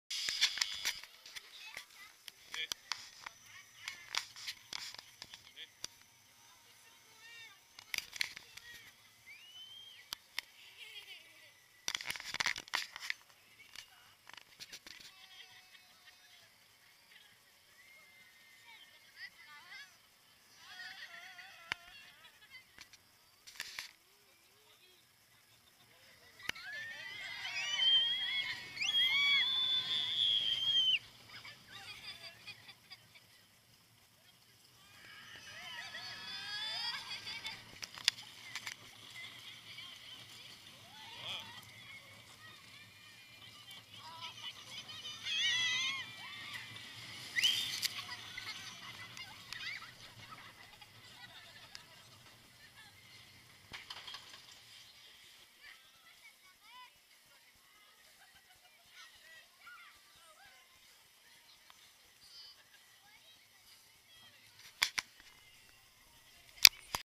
La B.R.A.P dans le "ROLLER-SKATE"
Vidéo prise avec tout le groupe présent ce jour à Plopsaland .
Joli bisou de Mcfly et Marjo au début ;-)